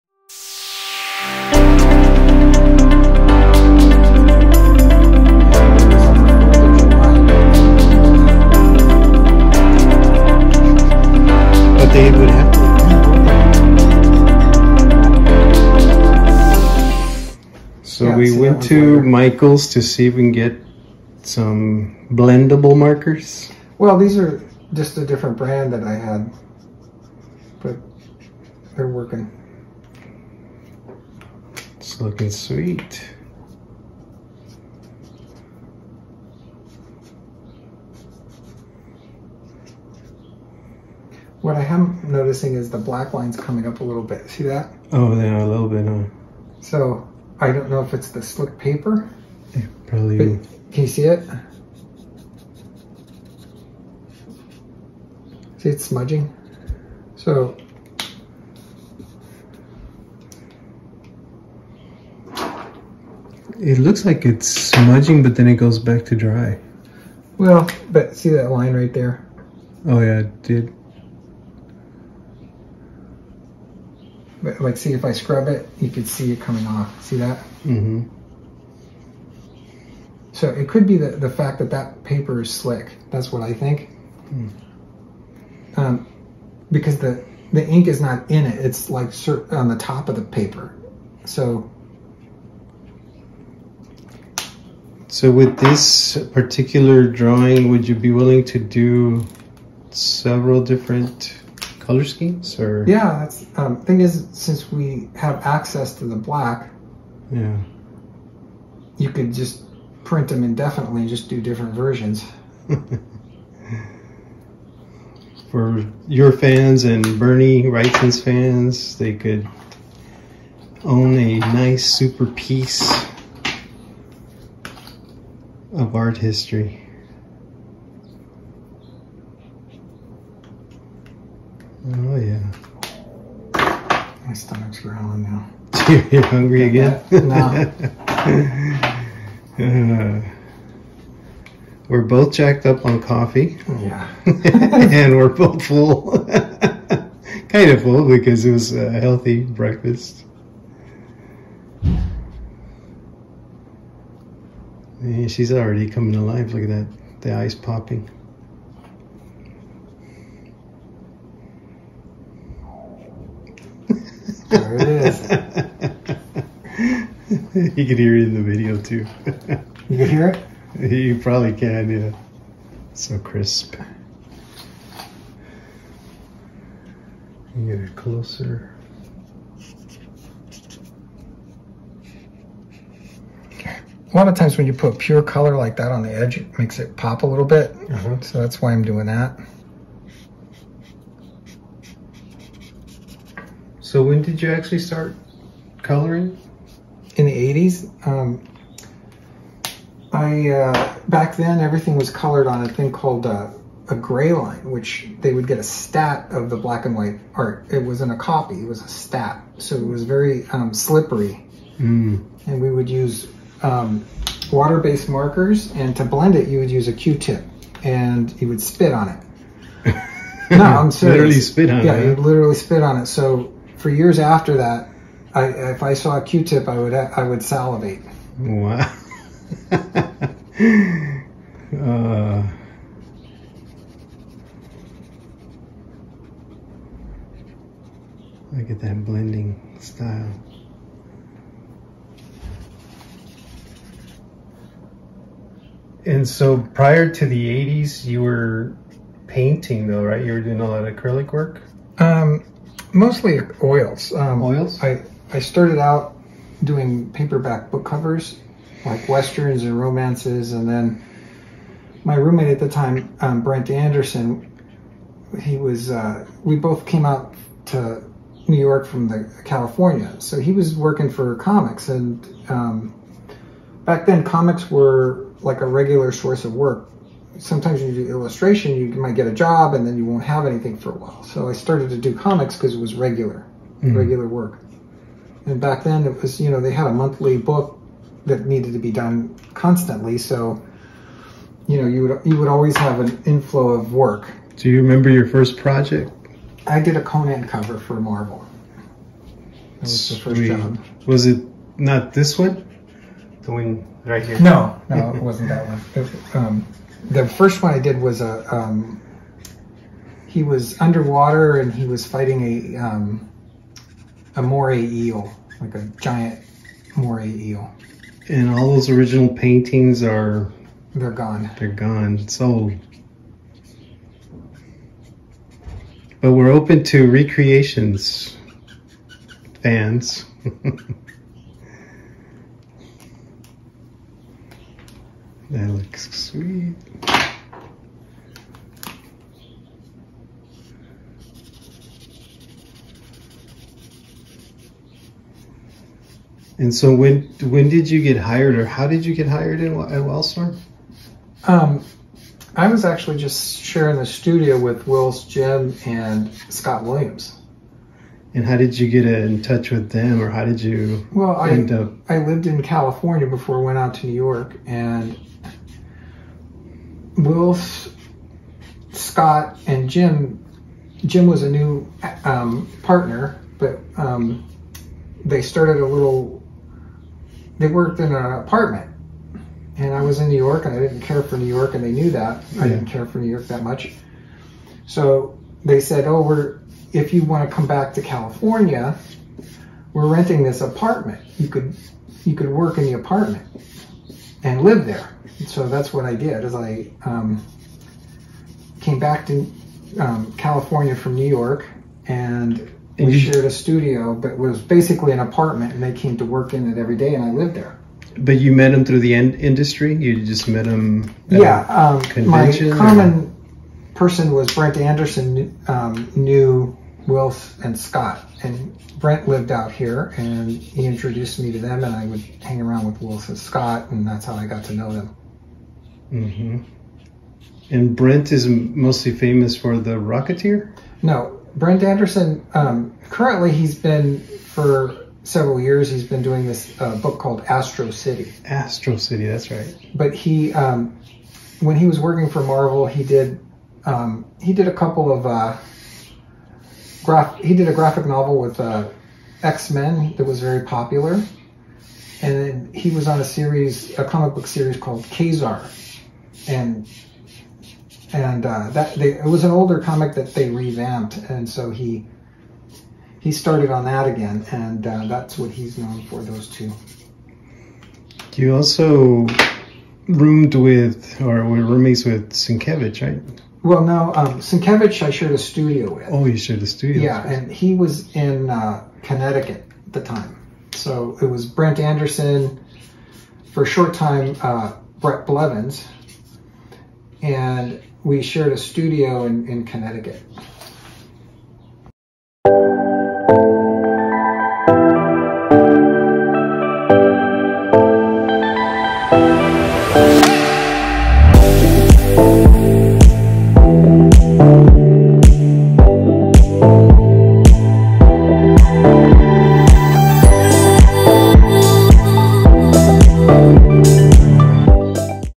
Chiodo over Wrightson Part 2
Master painter/artist Joe Chiodo continues to color a Berni Wrightson piece while he describes the process, his history in art, and people he met and worked with while in the business. original art,